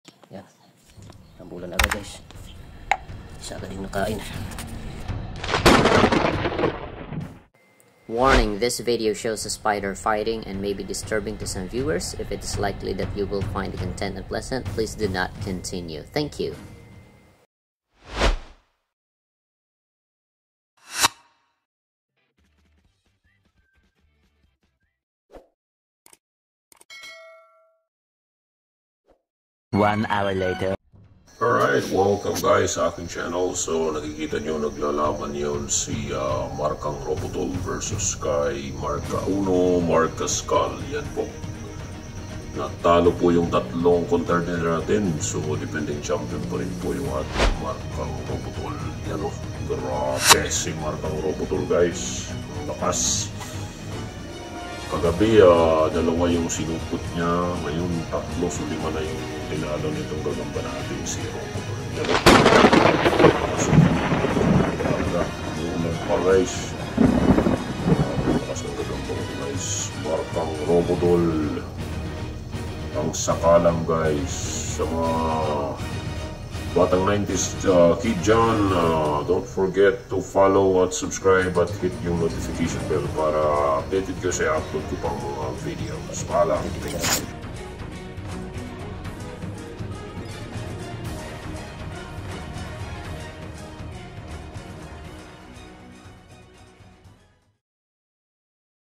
💥RUBOTOL VS '3 SPIDERS🔥
Just want to share my favorite hobby spider fighting.

G-Cash (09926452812) Mark Anthony B. Verzosa